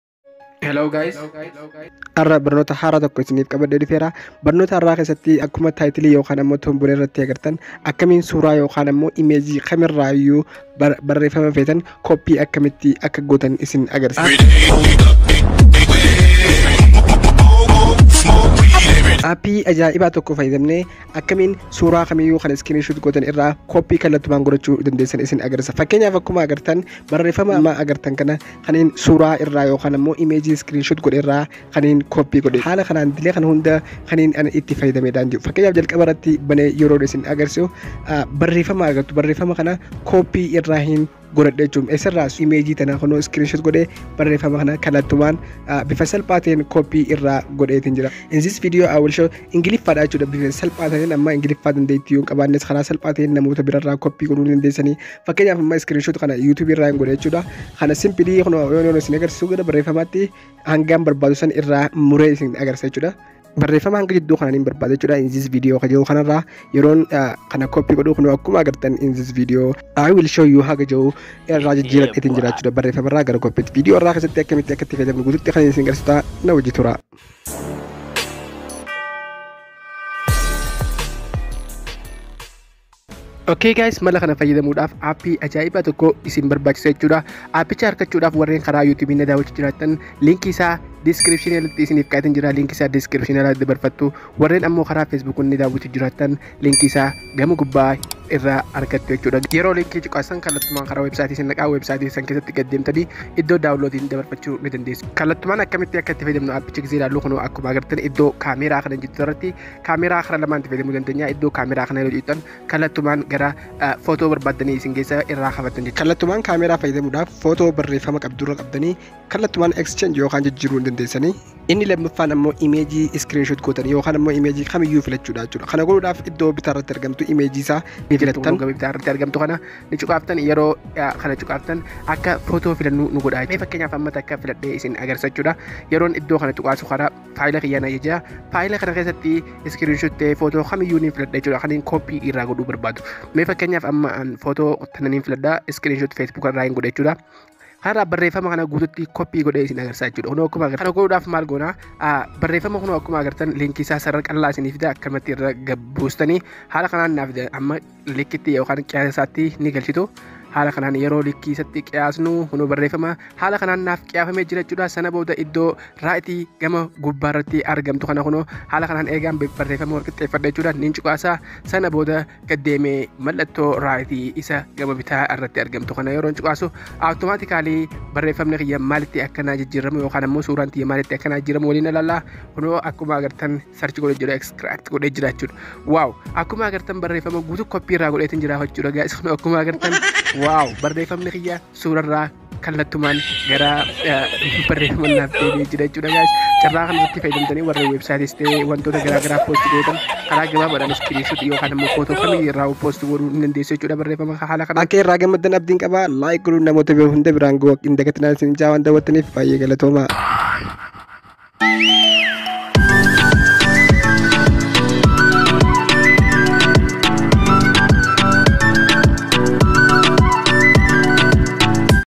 Barnoota Ajaa'ibaa: Akkaataa Suuraa Irraa Barreeffama copy itti goonu | How To Copy Text From Image
Guyyaa arraa barnoota akkaan isin gammachiisu akkaataa Image kamiyyuu irraa Barreeffama barbaanne Copy itti goonu isin barsiisa.
Akkaataa kanaan Barreeffama kamiyyuu afaan oromo tahee afaanota kamiyyuu Copy gootanii suuraa kamiyyuu irraa ykn Barreeffama suuraa screen shoot goone irraa laaftumatti Copy gootanii website tahee iddoo biraati fayyadamuu dandeessu.